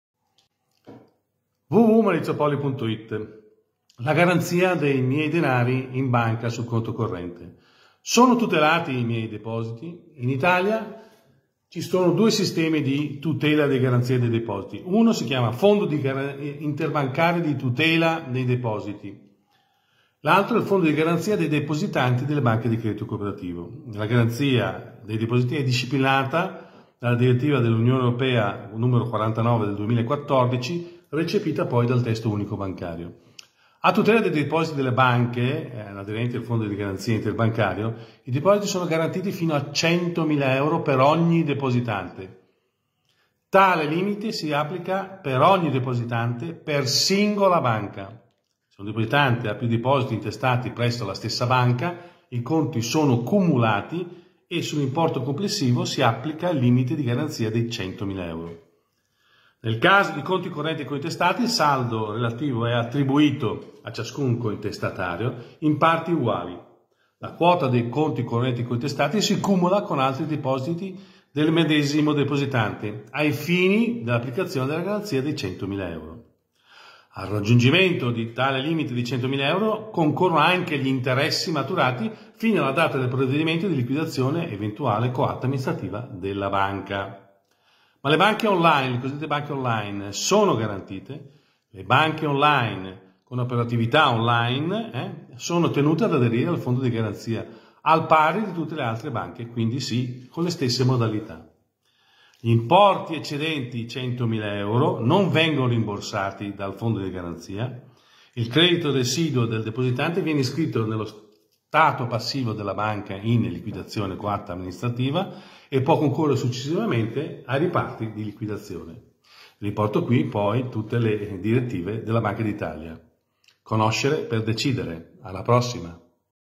La tutela dei depositi bancari : I depositi bancari sono tutelati ?
I depositi sono garantiti dal FITD fino a 100.000 euro per ogni depositante.
Tale limite si applica per ogni depositante, per singola banca. dettaglio
Maurizio Poli Educatore e Consulente Finanziario Certificato
Efpa Italia ANCP - Associazione Nazionale Consulenti Patrimoniali AIEF